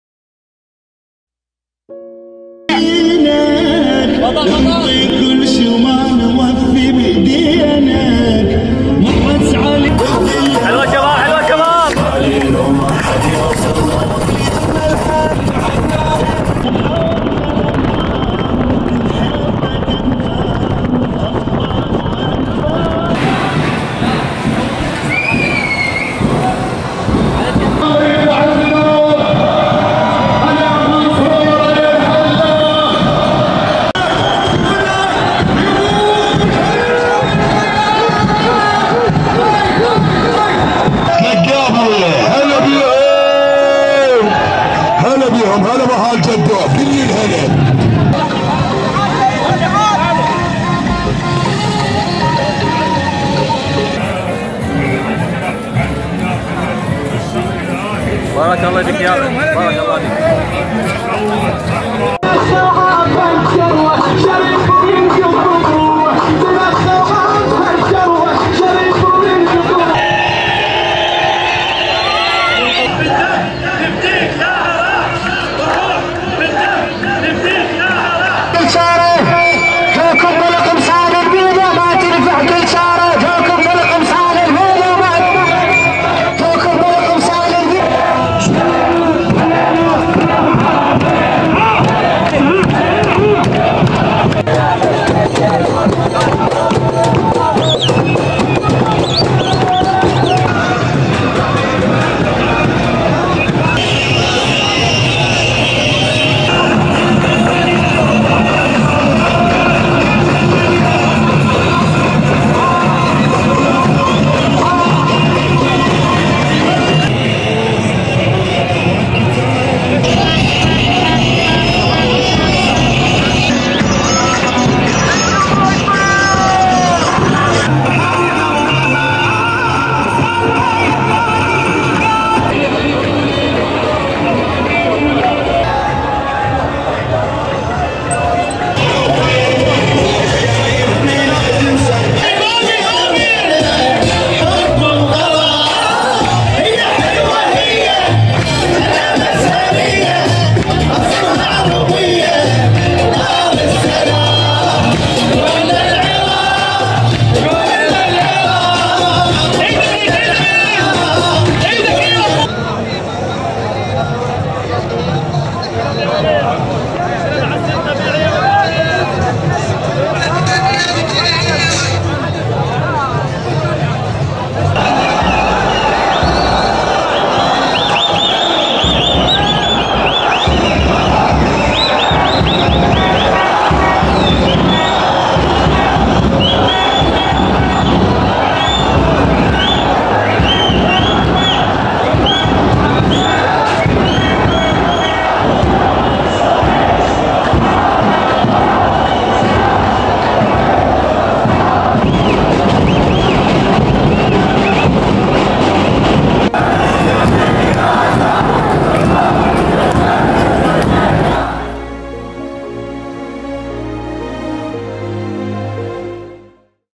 ثورة تشرين اليوم الرابع في محافظة بابل
تغطية ثورة تشرين اليوم الرابع في محافظة بابل

يوم جديد من ثورة تشرين اليوم انضموا ويانة اصدقائنا و اخواننا الطلاب
يوم كلش حلو جان , زيادة بأعداد المتظاهرين بشكل خيالي
التنظيم و جهود الشباب للمساعدة جدا رائع
مشاركة العنصر النسوي ديزداد يوم بعد يوم
الكل كان يطالب بحقوقه المشروعة بصورة سلمية 100%

و كعادة شكر خاص  لجهود خواننا في الاجهزة الامنية و فرق الدفاع المدني و الاسعاف
و شكر ايضا لجهود اخواننا في المواكب الحسينية الي مقصرو في توزيع الاكل ومياه الشرب و غيره

اخواننا في باقي المحافظات انتم في القلب يومية ادخل على صفحات اصدقائي و اشوف وضعكم.
اخواننا في بغداد استمرو كلنا وياكم و كلها تفتخر بيكم و كلنا دنتابع مغامرات الجسر و المطعم التركي.

و مننسى اخواننا الشهداء الرحمة و الخلود الكم ابسط شي نسويه كرد جميل هو نستمر بالمطالبة بحقوقنا المشروعة

يا عالم منريد شي بس